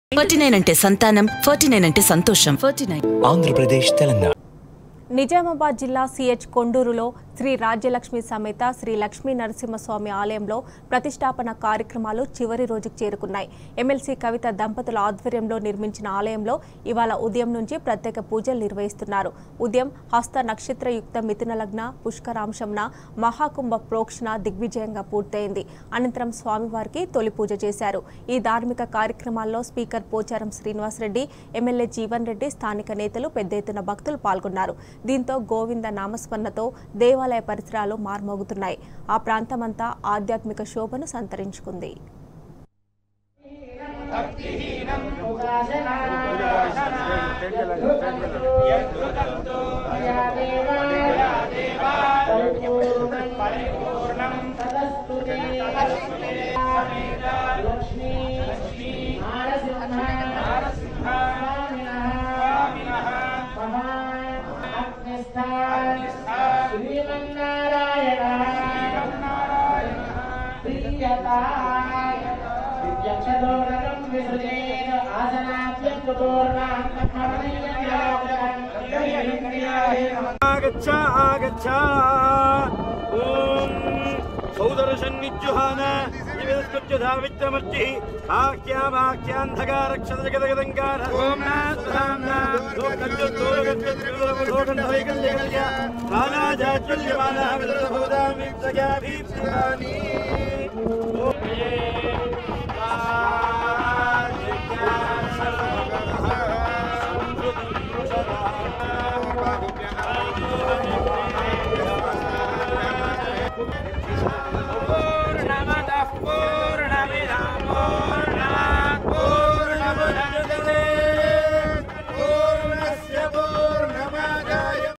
MLC Kavitha & Family Participates in Sri Laxmi Narasimha Swamy Temple rituals for 6th Day | T News
MLC Kavitha & Family Participates in Sri Laxmi Narasimha Swamy Temple rituals for 6th Day |